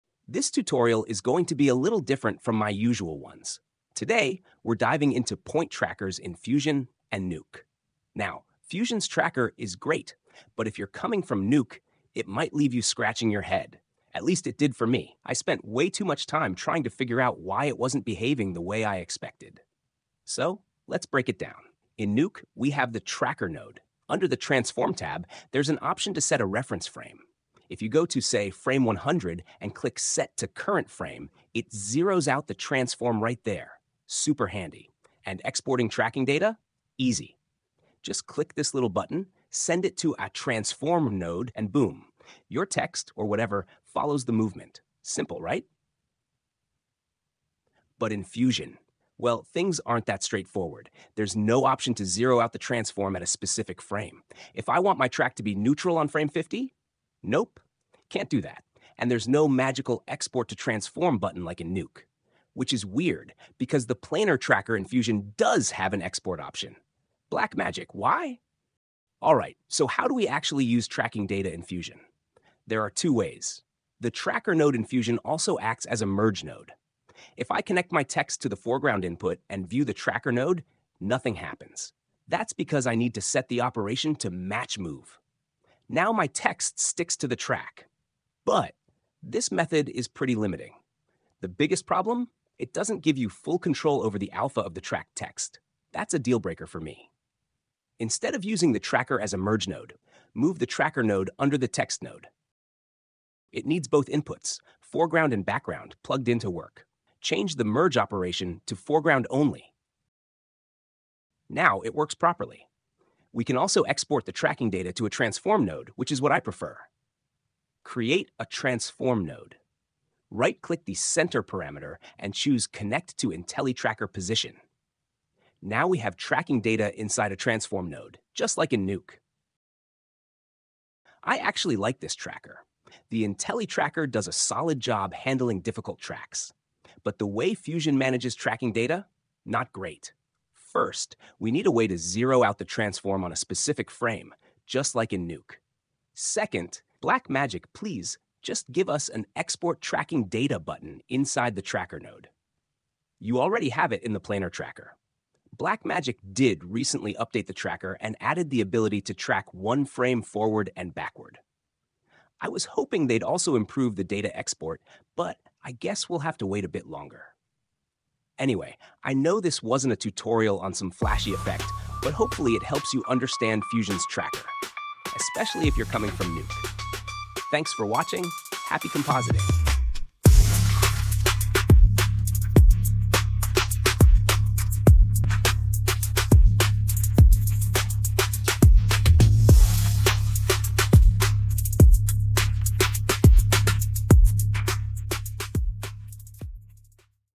Nuke vs Fusion: Understanding Point Tracker in Fusion
Fusion’s point tracker is powerful, but if you’re coming from Nuke, it might feel a bit… confusing. In this tutorial, I break down the differences between the two, show you the best way to use Fusion’s tracker, and explain why Blackmagic needs to add a couple of simple (but crucial) features.

Topics covered:

- How Nuke’s tracker works vs. Fusion’s tracker
- The "bad" way to use Fusion’s tracker
- The better way to work with tracking data in Fusion
- Export tracking data to a transform node

If you're a compositor switching from Nuke to Fusion (or just curious about tracking workflows), this video is for you!